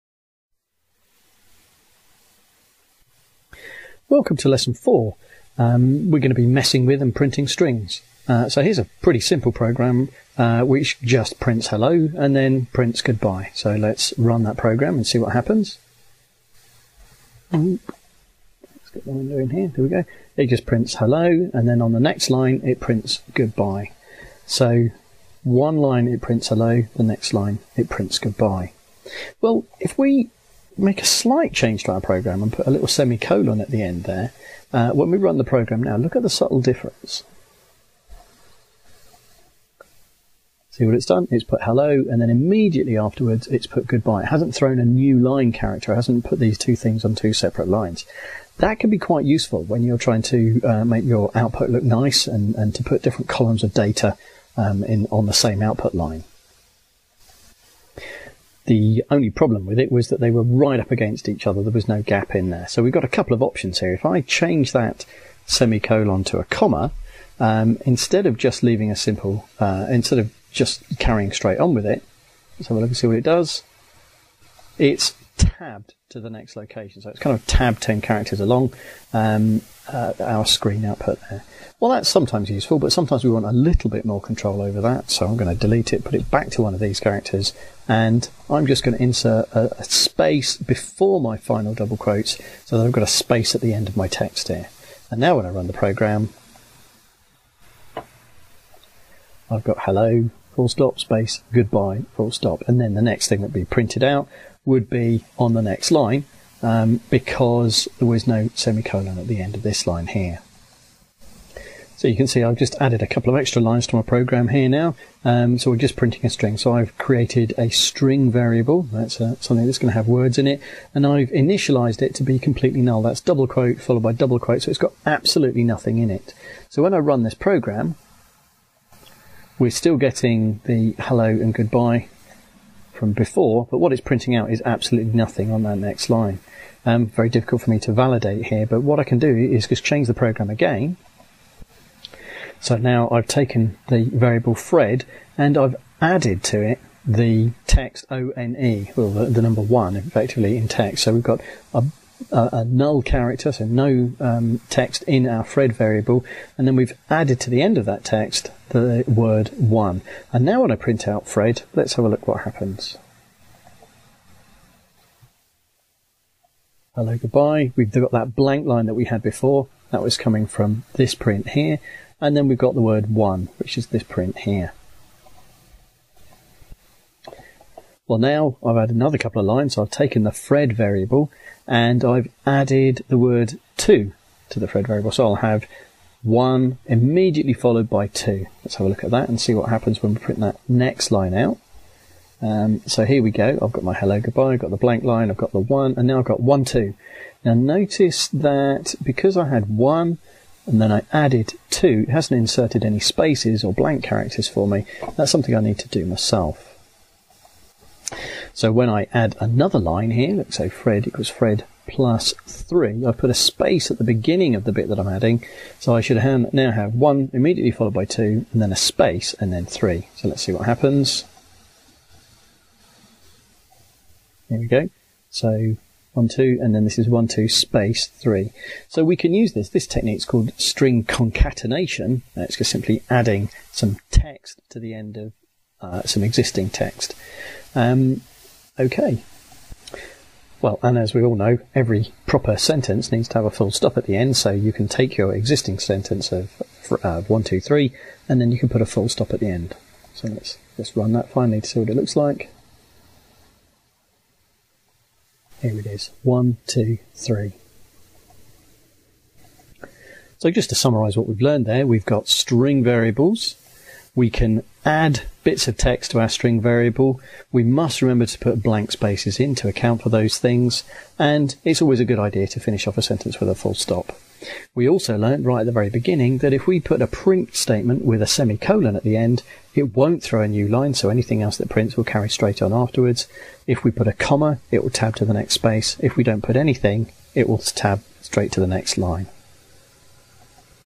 The basics of QBasic - Lesson 4 Concatenating strings and tidying up output
In this QBasic programming skills clip, you see a few simple techniques to help you build up your output message using string concatenation and some of the special print characters.